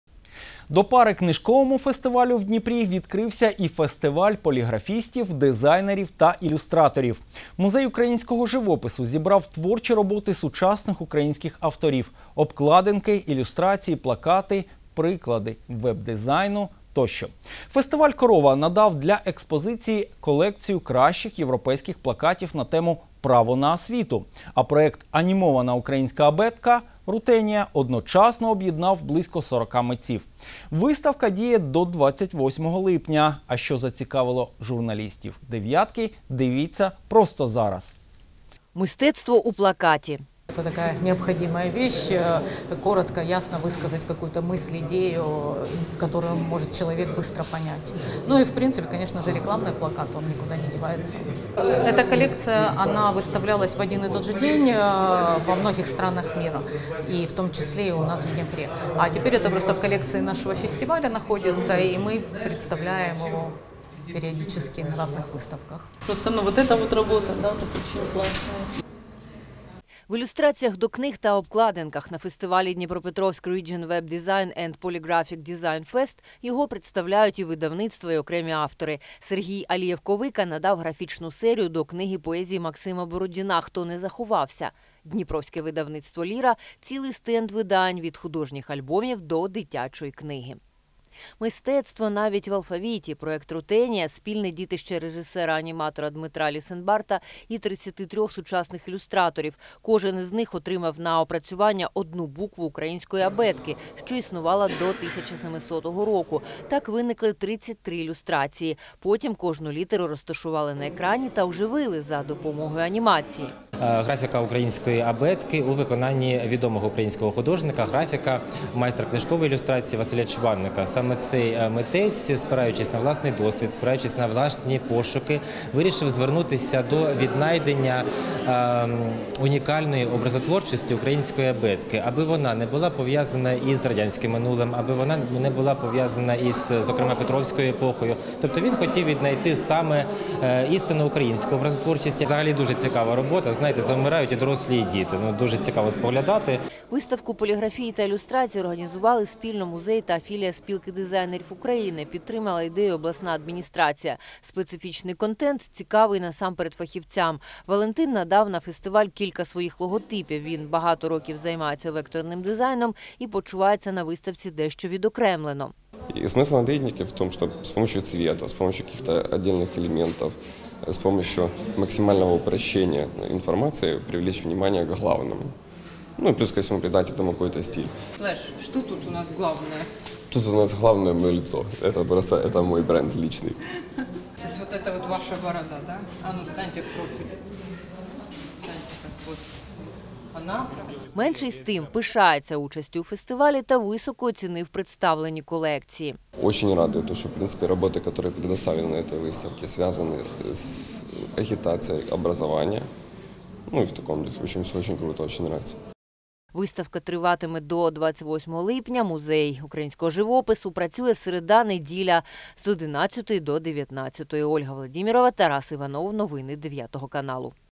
Dnipropetrovsk region web-design&polygraphic design fest - 2019 у Дніпрі
Dnipropetrovsk region web-design&polygraphic design fest - 2019 розпочався в Музеї  українського живопису.
Музей українського живопису та Дніпровська філія «Спілки дизайнерів України» за ініціативи Дніпропетровської обласної державної адміністрації презентували: Dnipropetrovsk region web-design & polygraphic design fest – 2019 - виставку робіт ілюстраторів, художників-графіків, поліграфічних дизайнерів.